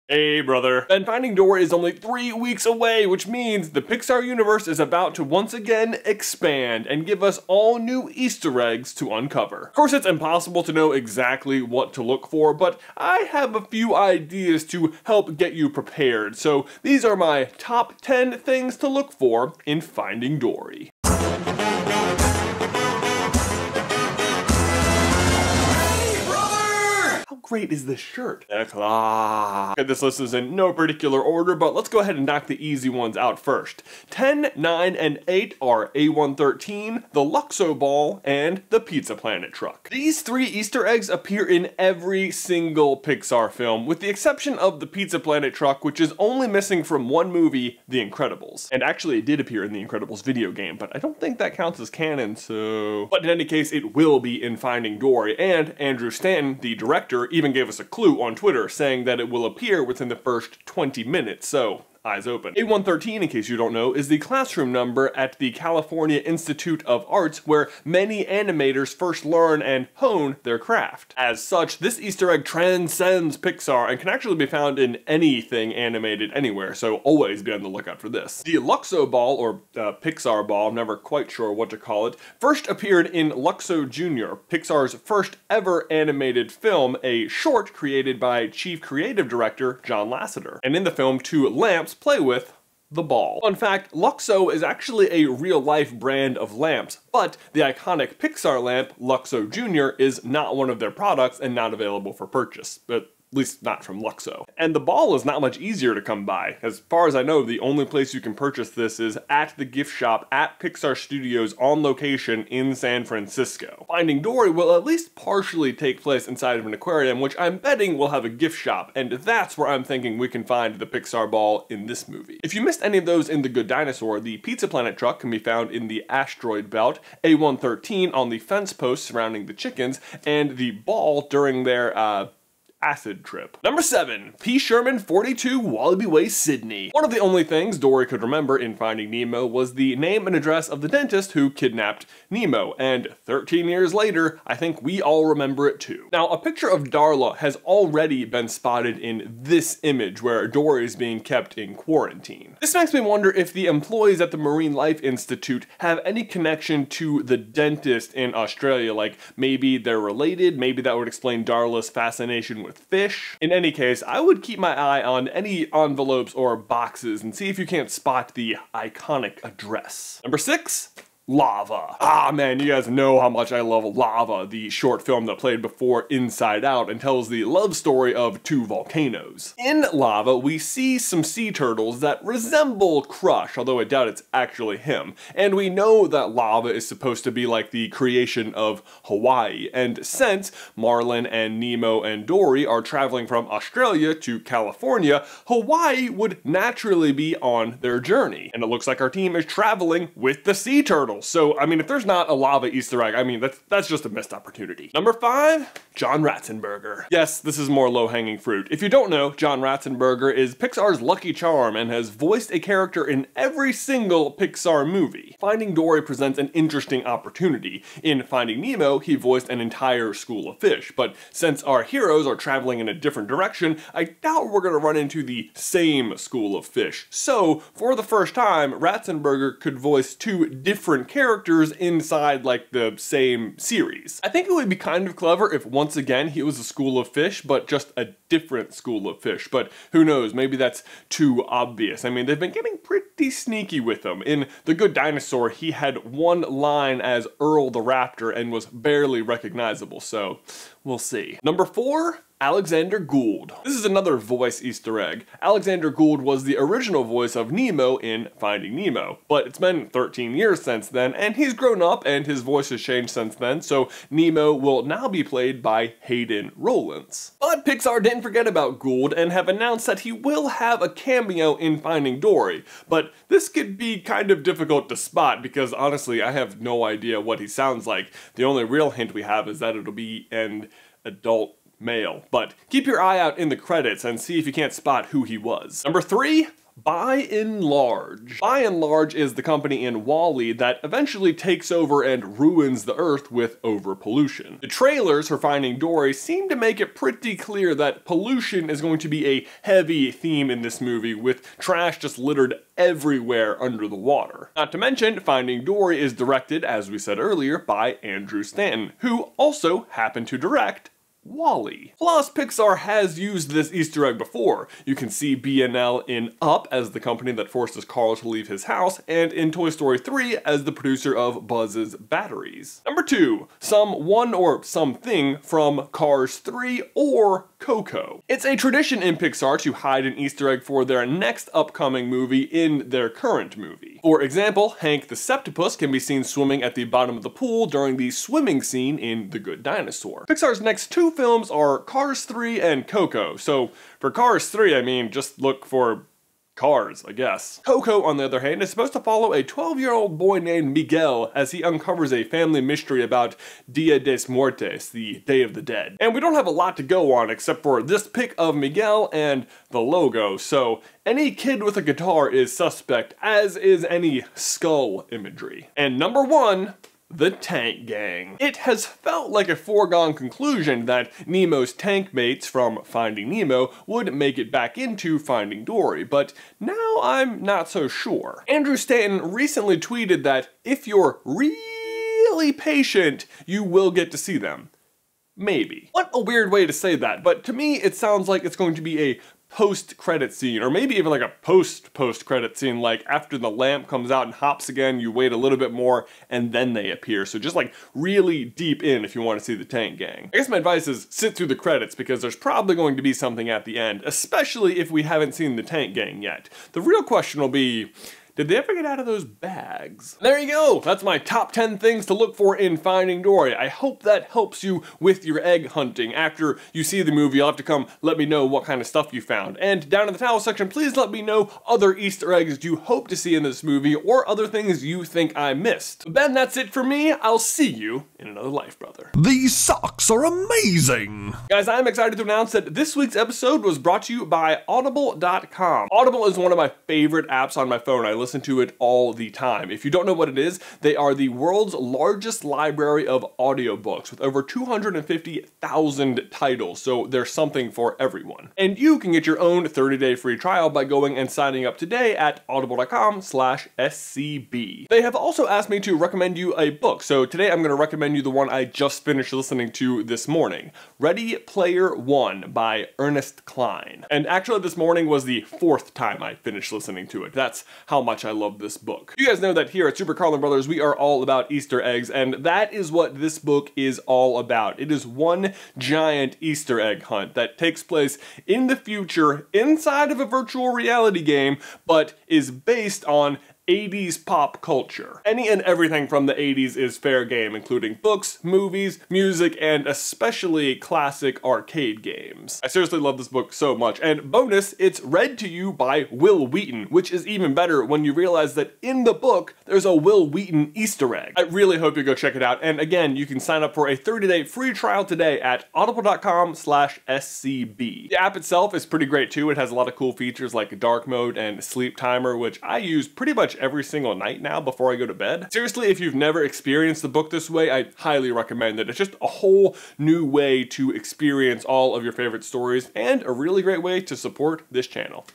Top 10 Things To Look For In Finding Dory
J makes predictions about the top 10 easter eggs you should be looking for when Pixar's Finding Dory hits theatres in June!

Every time you give us a thumbs up a Narwhal gets its horn! (maybe..) =D